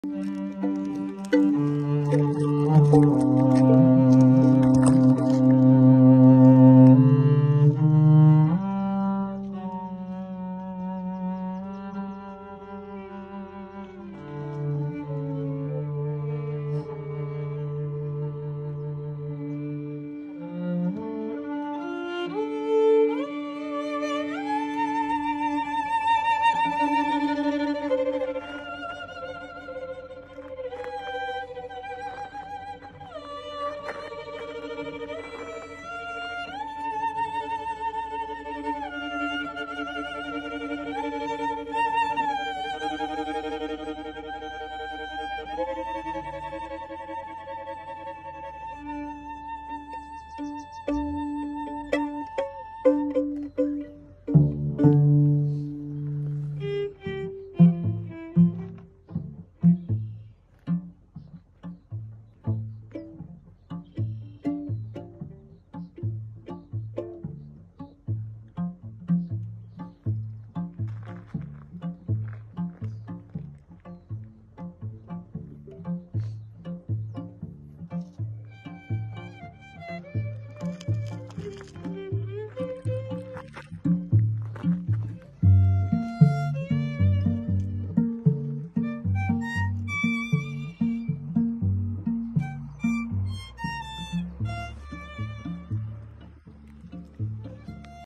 Strings in the Woods at Santa Fe Canyon with Cellist at Sunset at Pond
Strings in the Woods New Mexico
Check dates here: Santafe.StringsintheWoods.com
The Cello Player, Will Taylor, Santa Fe Canyon